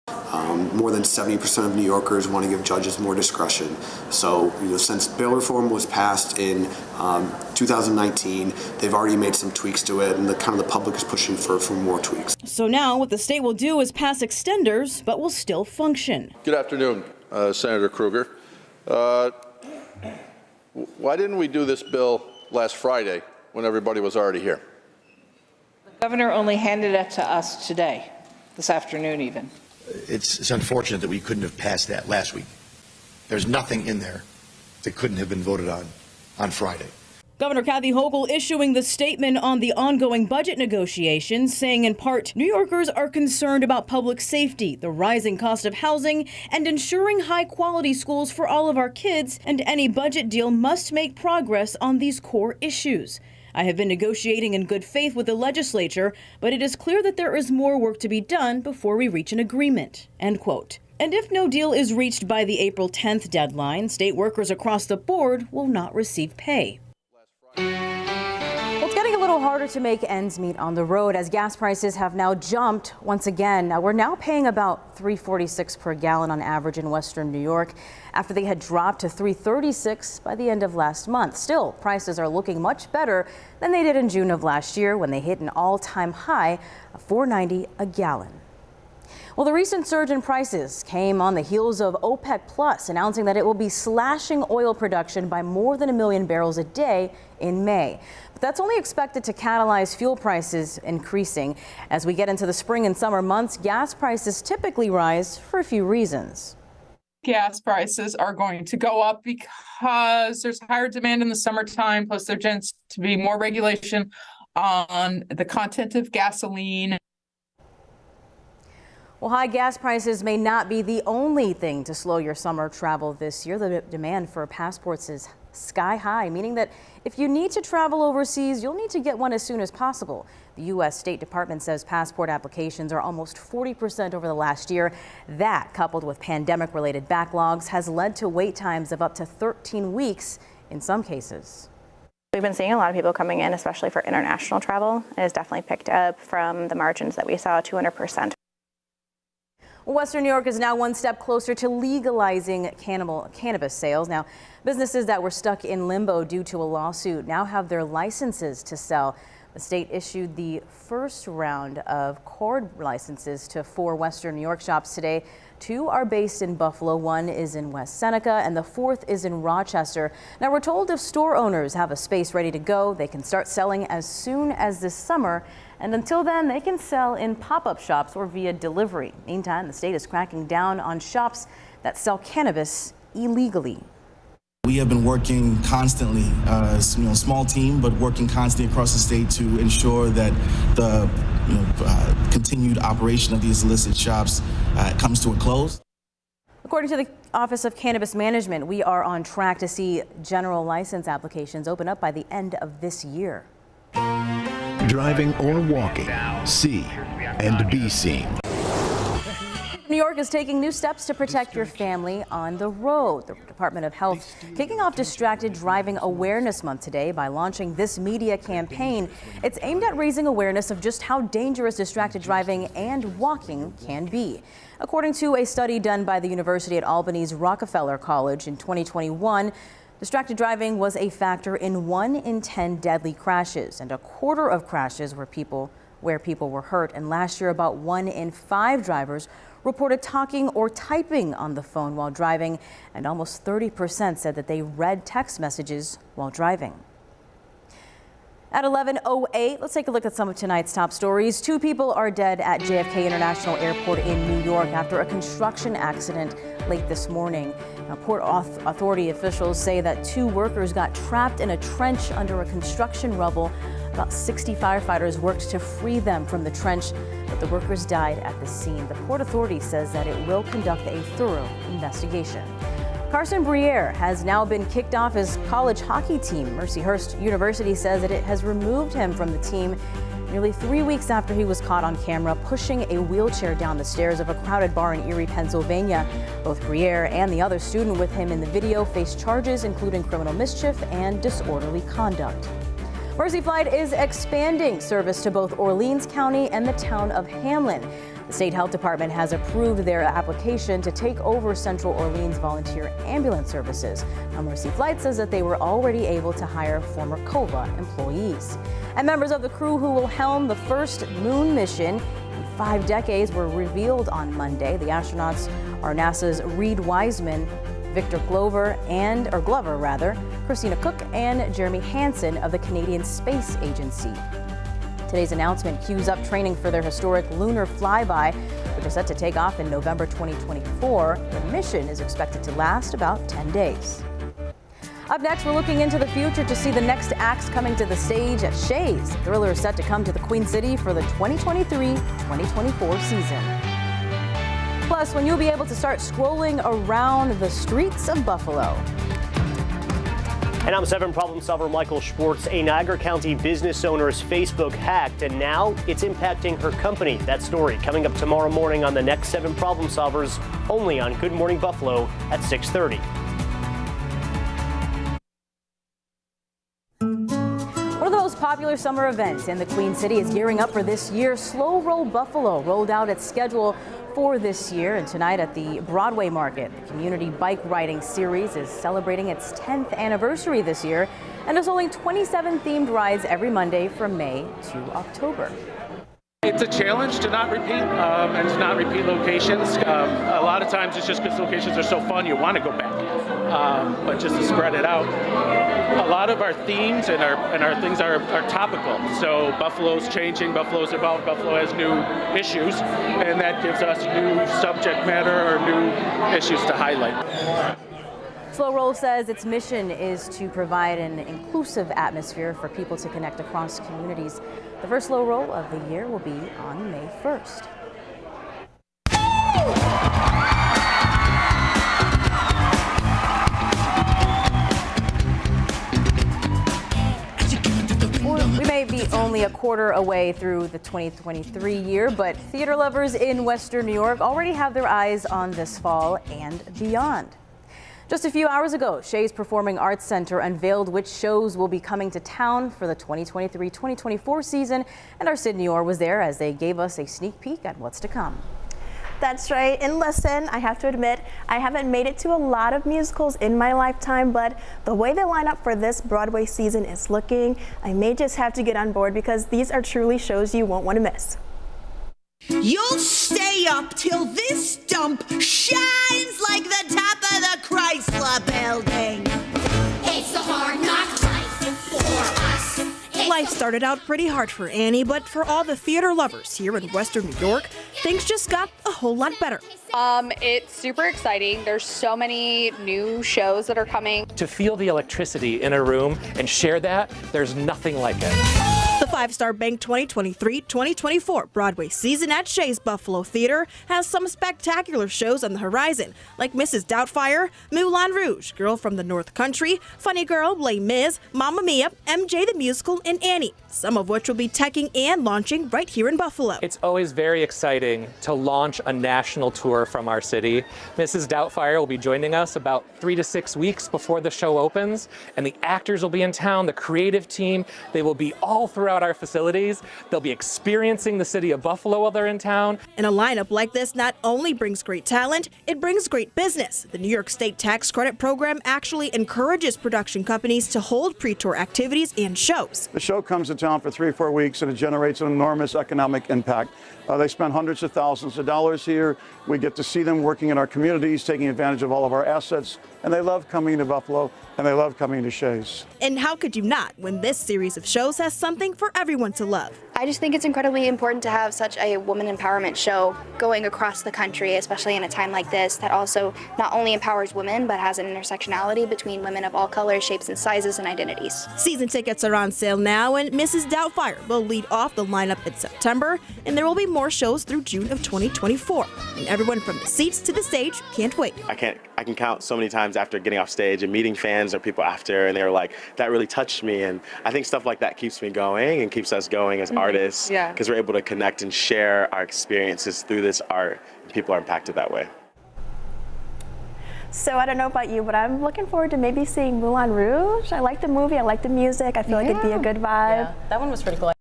Mrs. Doubtfire, Moulin Rouge! lead off 2023-24 Broadway season at Shea's Performing Arts Center
Two blockbuster movies turned musicals makeup part of a 'thriller' of a lineup coming to Shea's Performing Arts Center in Buffalo.